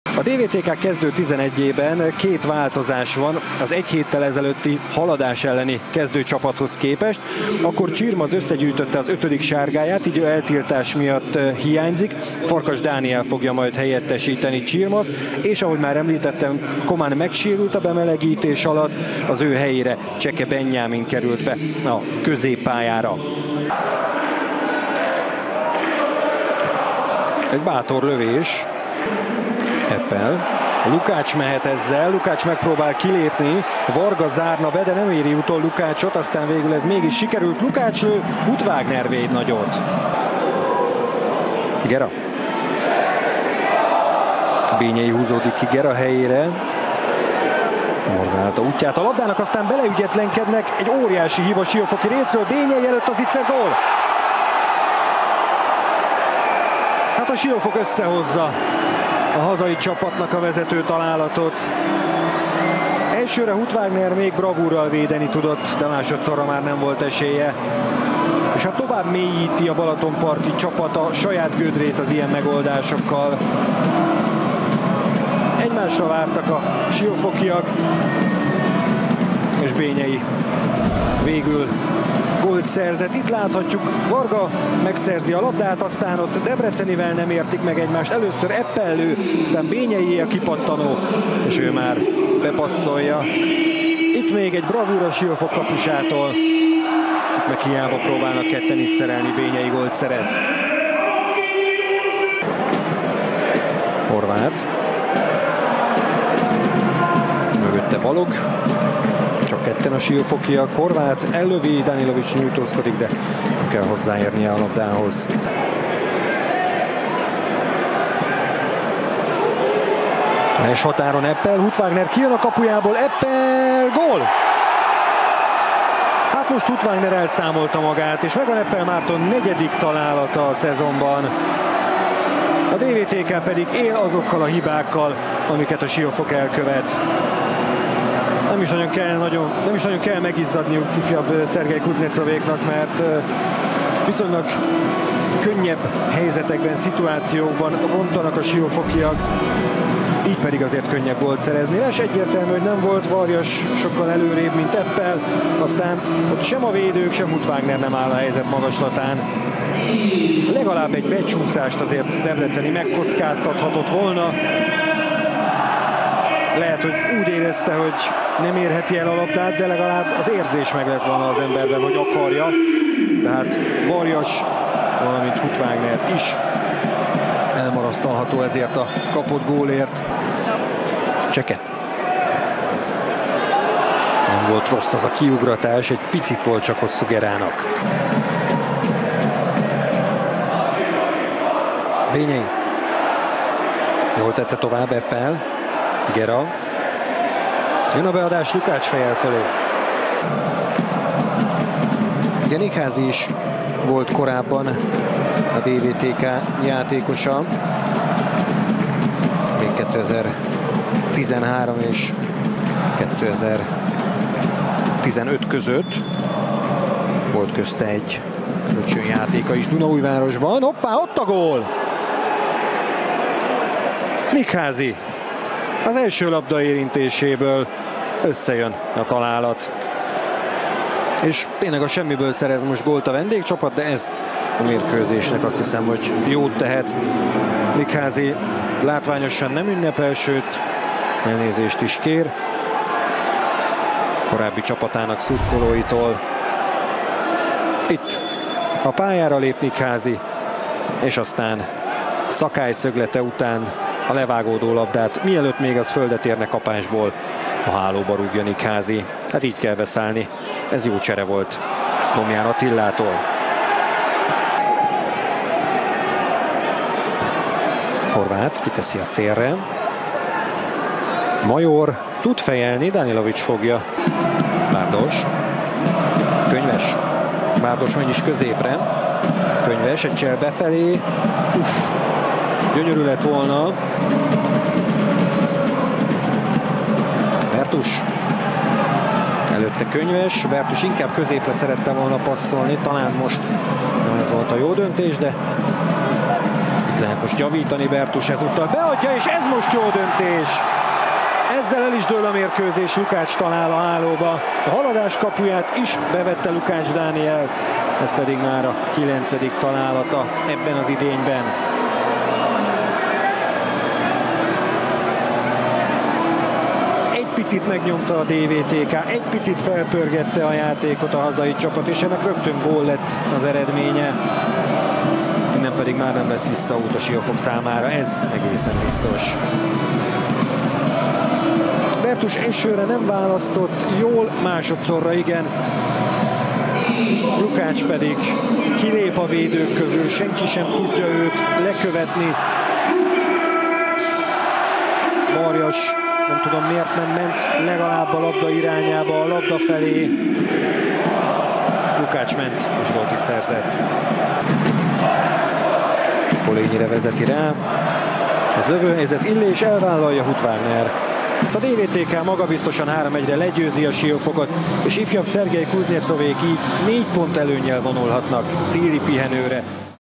DVTK - Siófok összefoglaló | 2022.12.11. | Merkantil Bank Liga
Merkantil Bank Liga 20. forduló

Diósgyőri VTK - BFC Siófok 3-1 (2-0)

Gólszerző: Bényei Ágoston (1-0) a 14., Eppel Márton (2-0) a 34., Nikházi Márk (2-1) a 60., Lukács Dániel (3-1) a 82., percben.